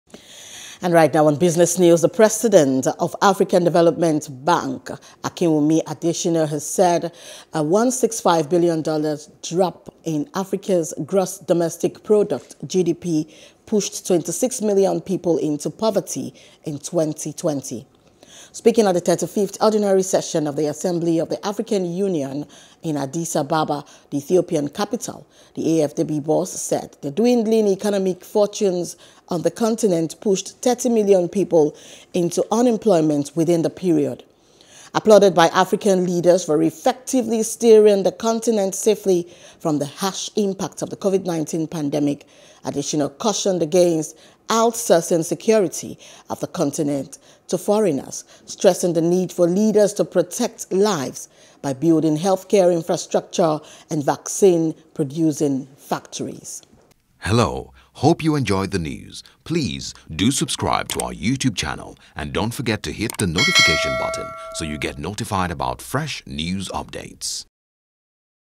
Africa’s G.D.P.: AfDB Laments Drop By $165b In 2020 | BUSINESS
The President of African Development Bank (AfDB) Akinwunmi Adesina has said a $165 billion drop in Africa’s Gross Domestic Product (G.D.P.) pushed 26 million people into poverty in 2020.